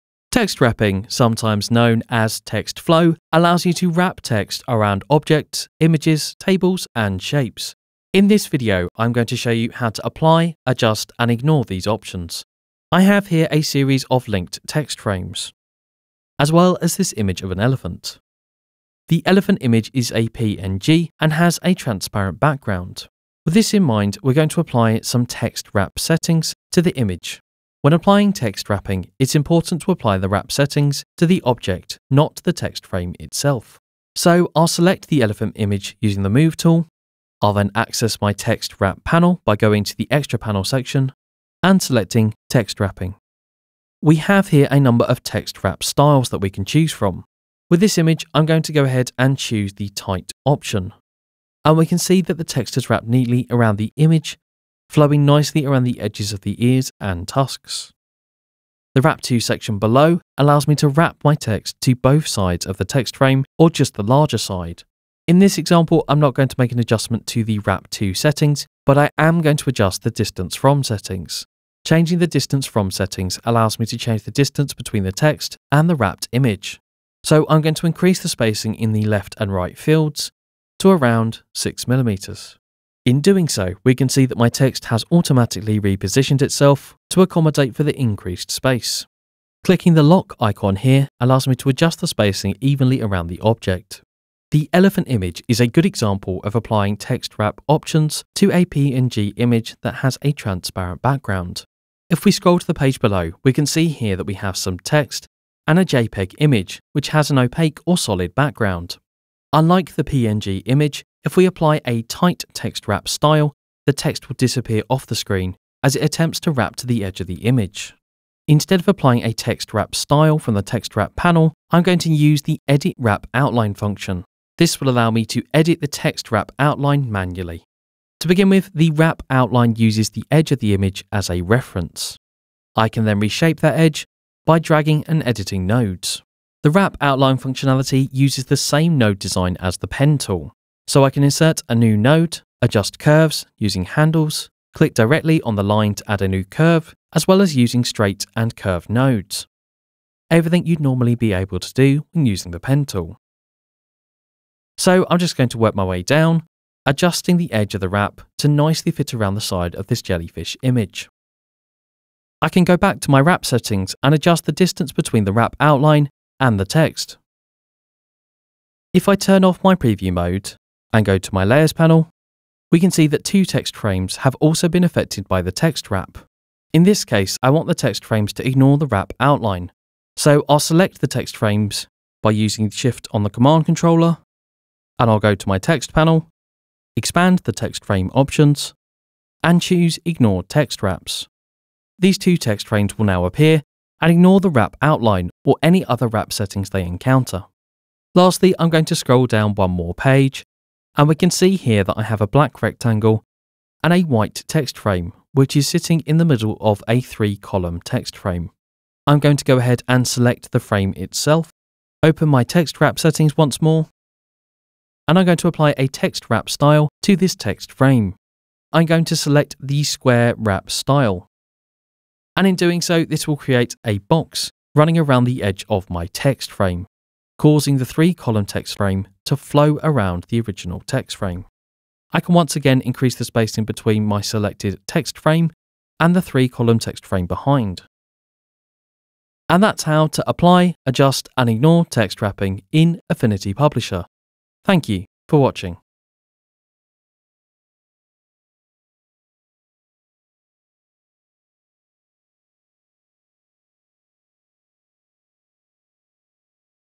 Text Wrapping (Affinity Publisher iPad)
Learn how text wrapping works in the iPad version of Affinity Publisher, including the ability to draw out your own non-destructive wrap outline.